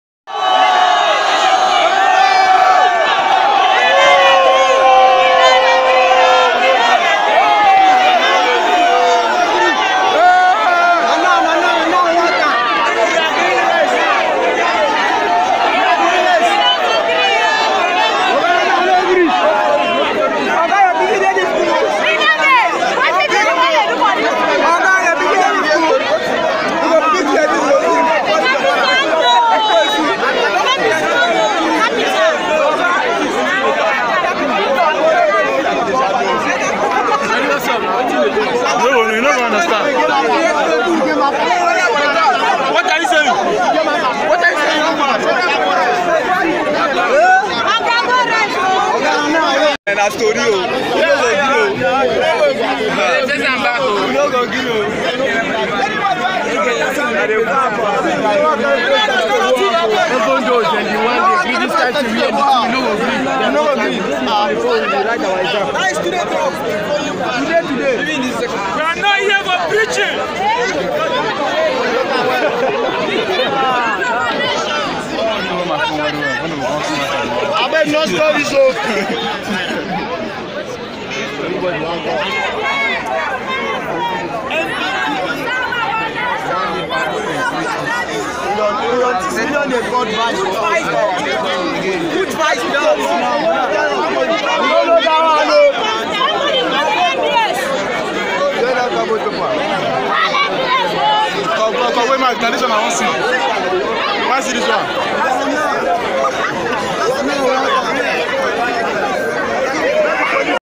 student protesting for their right, accreditation of course in nasarawa state polytechnic
student are angry with the school for not accrediting their course, result are not approved\.